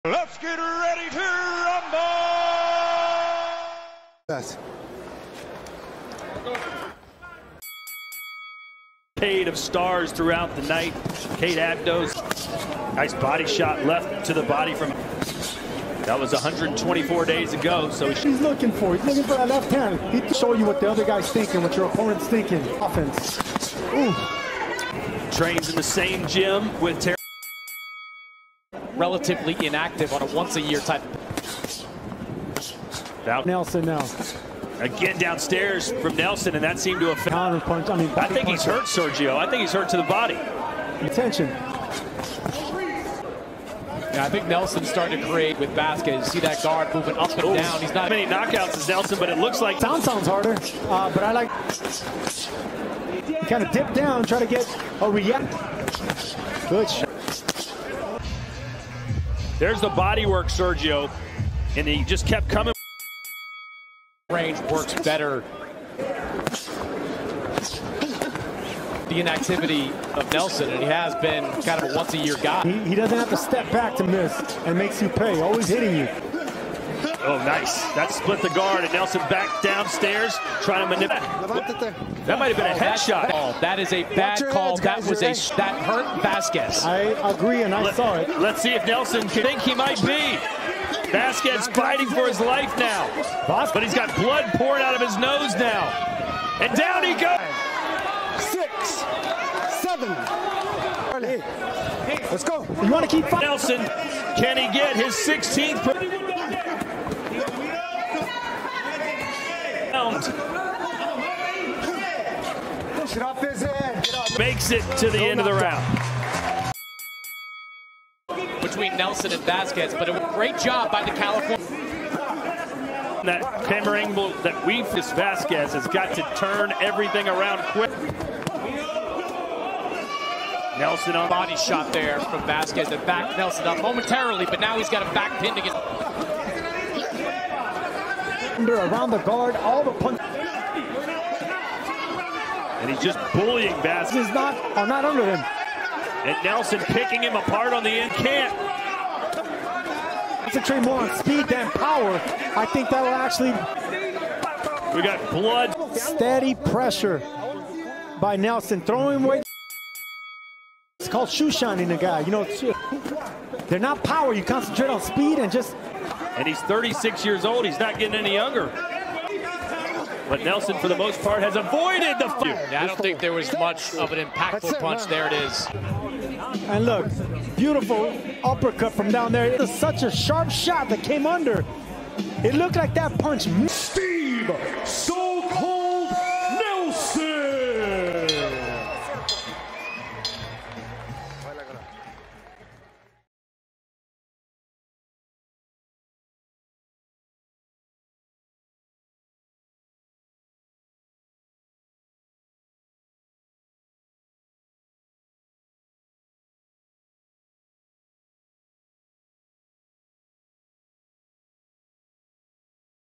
Steve Nelson vs Marcos Ramon Vazquez | Full Fight Highlights | BOXING FIGHT | HD
Watch the intense highlights of the bout between Steve Nelson and Marcos Ramon Vazquez! Witness the skill, power, and strategy as these two fighters face off in a thrilling match filled with dramatic moments and high-octane action. Relive the key exchanges and unforgettable highlights that make this fight a must-watch.